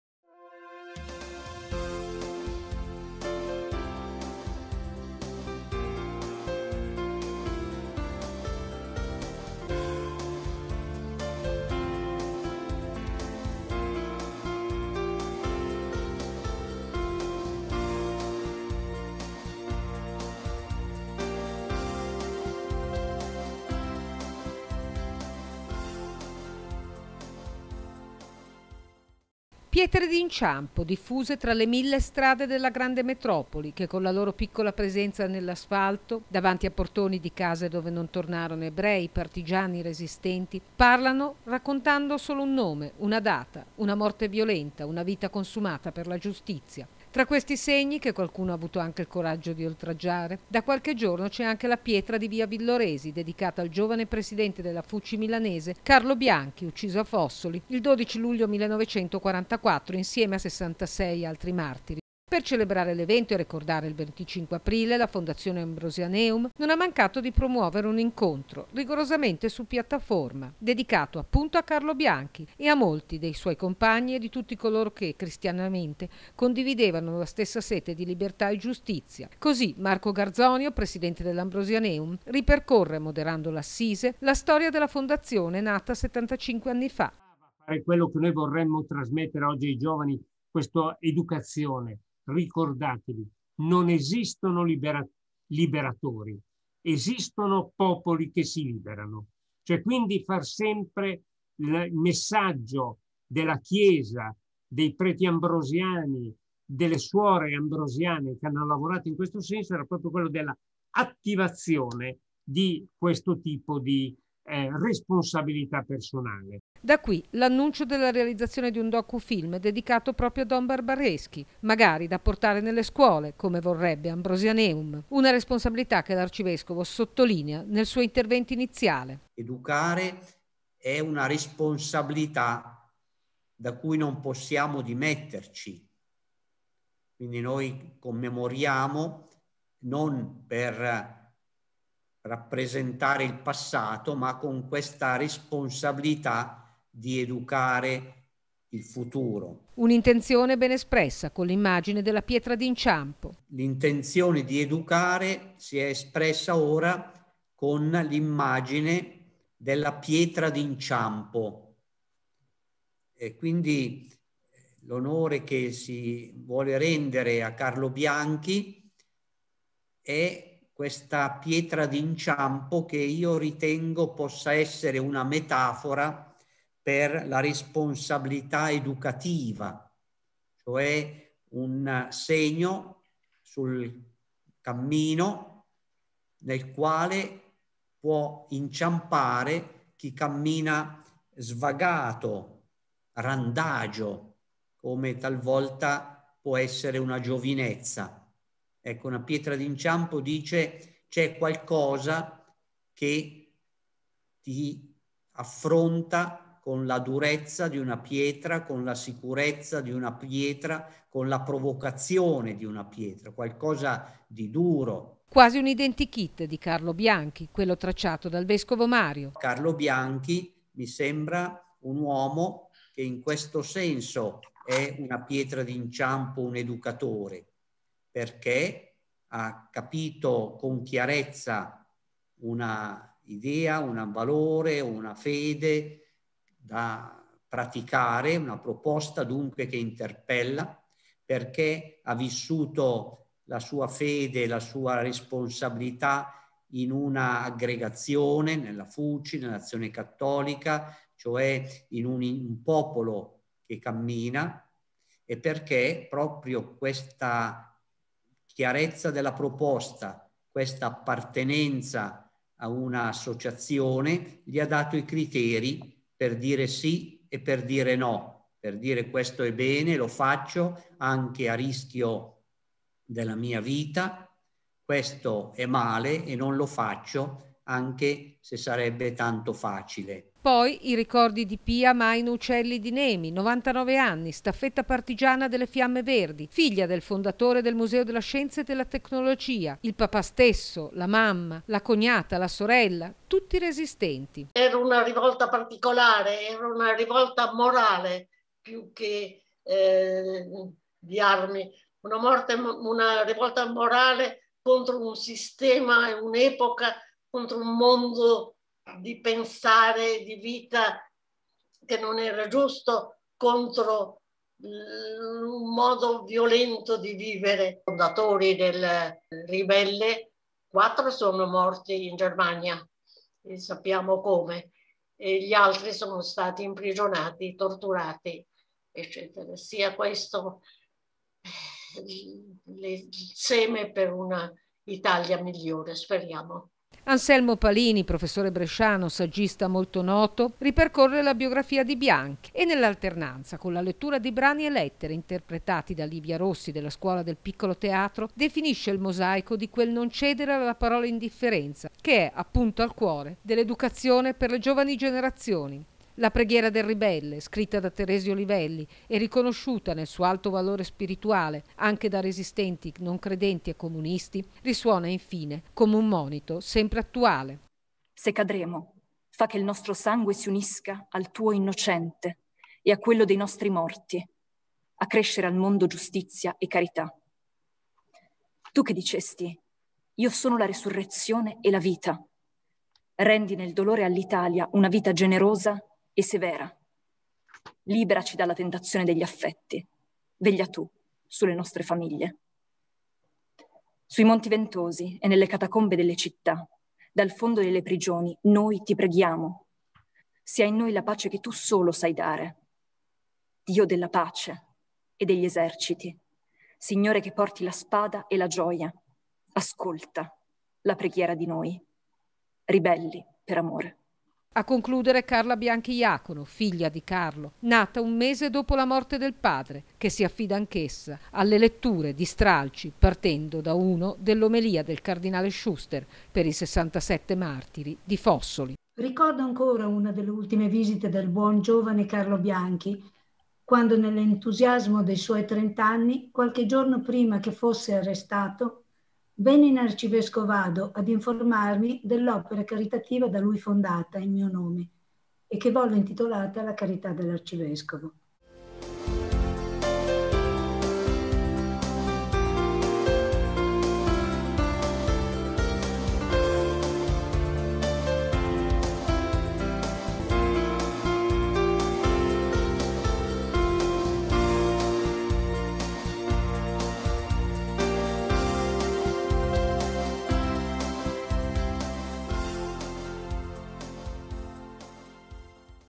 "Dalla Resistenza al futuro, il compito di educare". Servizio di "ChiesaOggi"
Il servizio giornalistico di Annamaria Braccini sul convegno organizzato in occasione della posa della pietra d'inciampo a Carlo Bianchi da Fondazione Ambrosianeum e Arcidiocesi di Milano, Servizio di Pastorale Giovanile diocesano, Azione Cattolica Ambrosiana, ACLI provinciali di Milano, Monza e Brianza, FUCI e Associazione delle Fiamme Verdi.